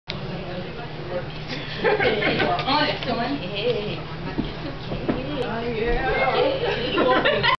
Got Some Cake Dance 2
baby cousin goes crazy for cake